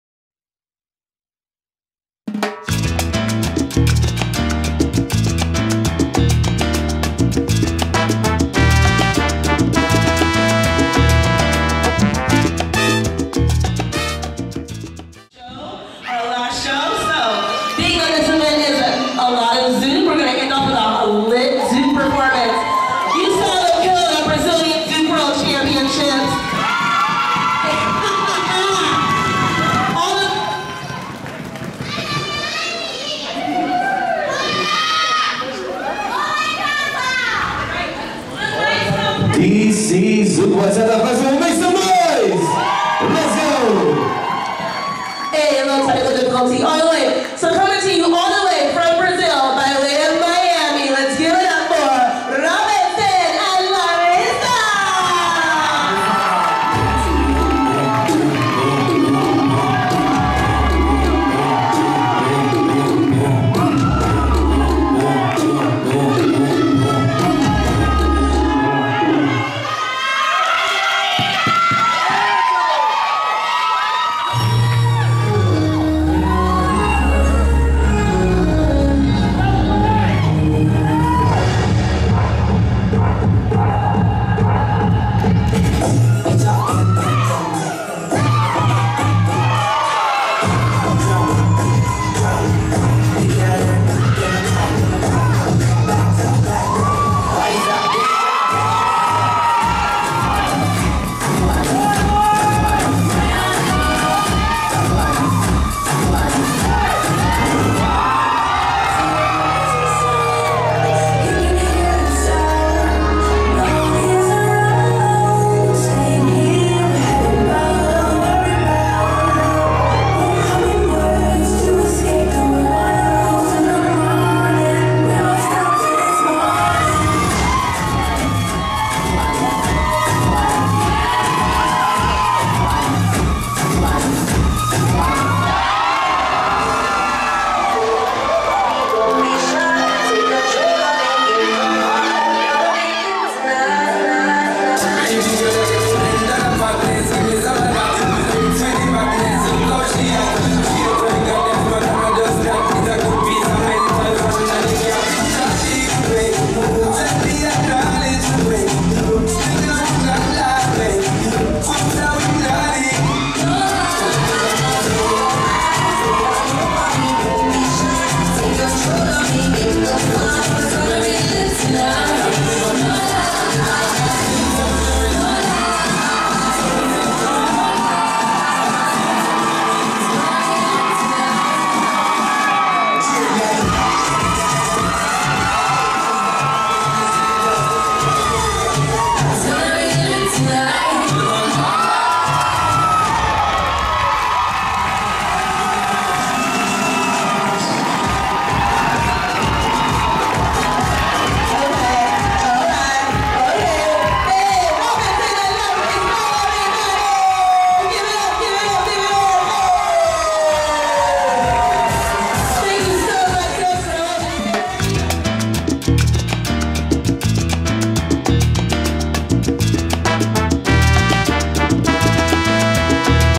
DC Zouk & Bachata - 6/10/2023 - Robinson & Larissa
Robinson & Larissa perform at DC Zouk & Bachata in Baltimore, MD.